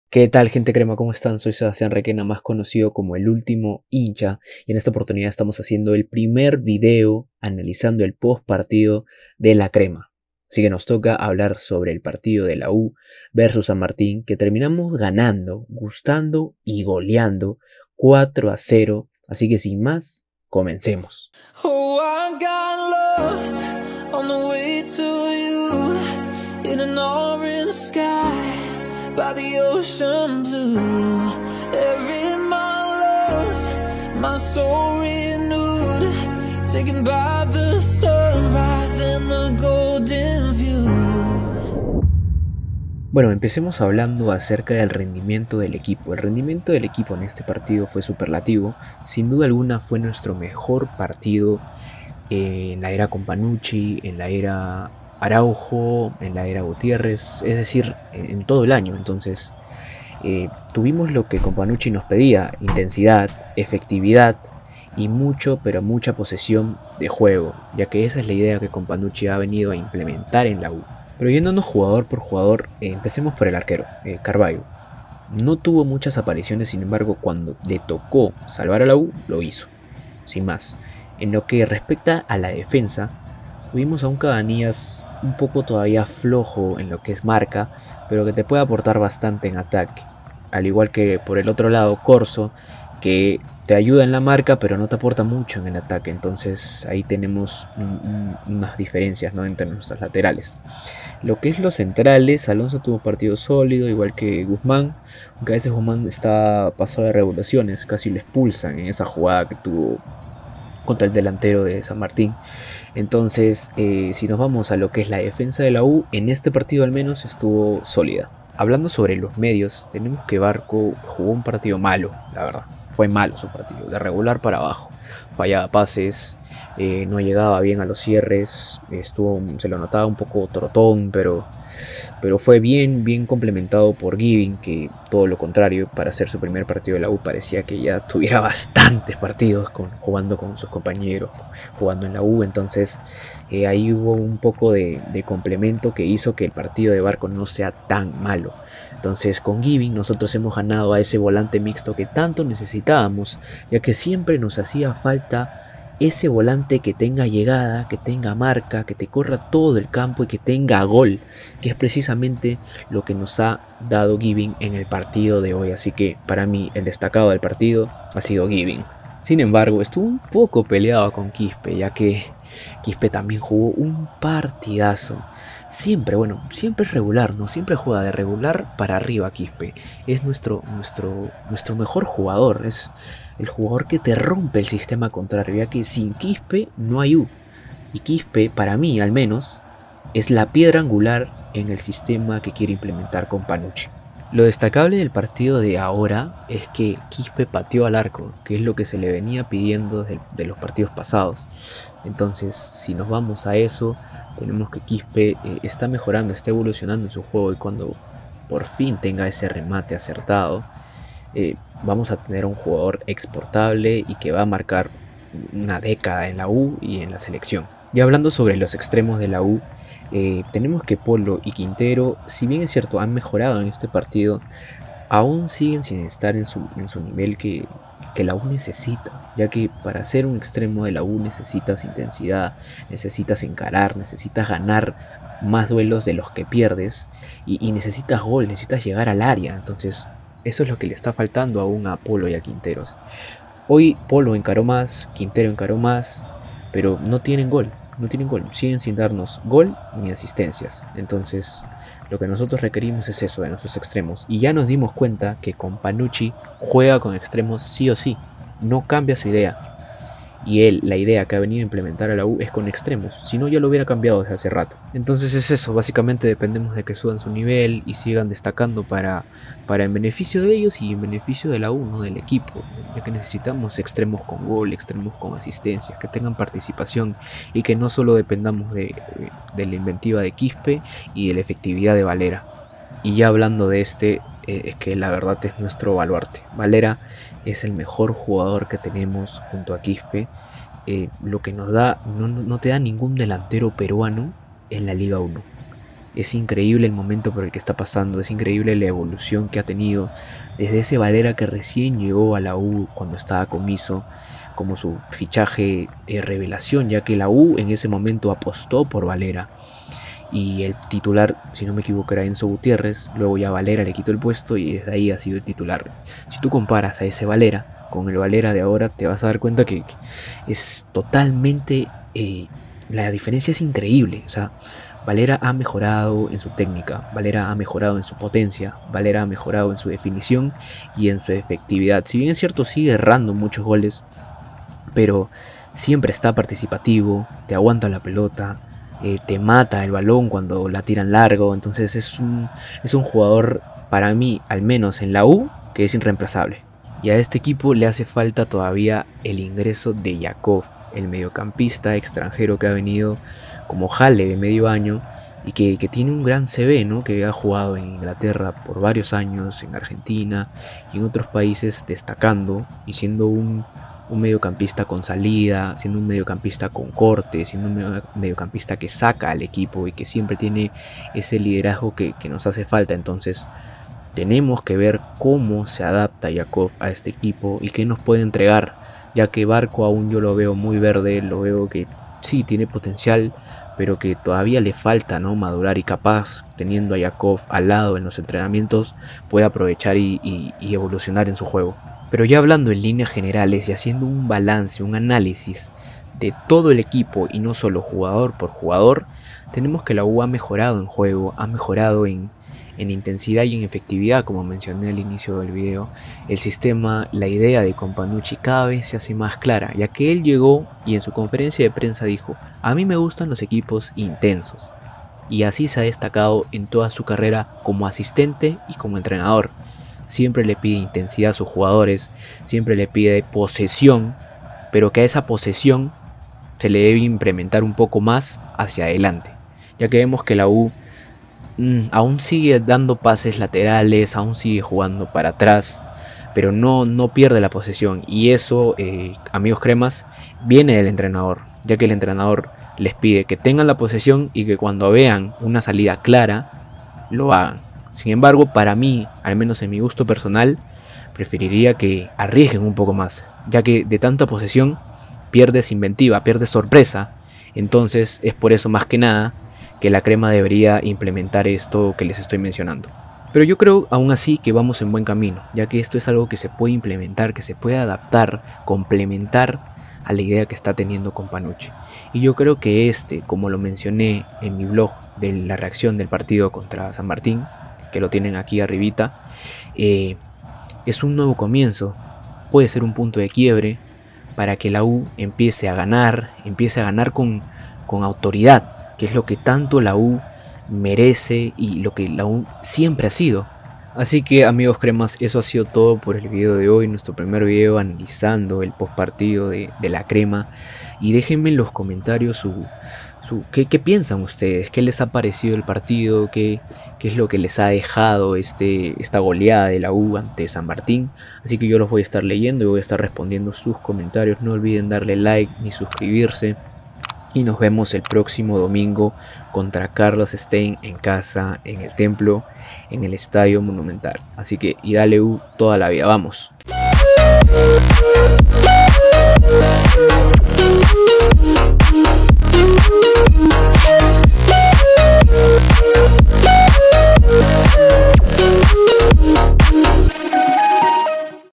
¿UNIVERSITARIO A PASO FIRME POR EL CLAUSURA? - ANÁLISIS POST PARTIDO
🎙 Analizando la victoria de Universitario de Deportes frente a la San Martín por la fecha 2 del Clausura. De crema a crema, está es mi opinión.